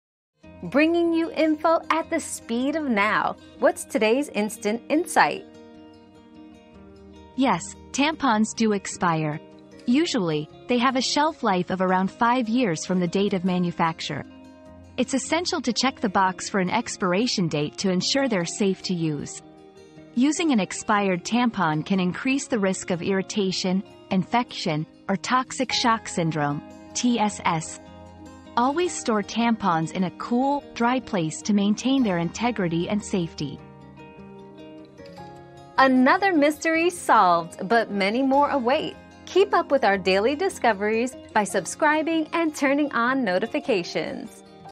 Do tampons expire?
Do Tampons Expire? Important Facts You Need to Know 👉 Tampon Expiration Facts 👉 Learn why tampons expire and the potential risks of using an expired tampon. Keep yourself safe by checking the expiration date and storing them properly.

Laura S. Harris (2024, April 9.) Do tampons expire?
    🌐 AskAbout.video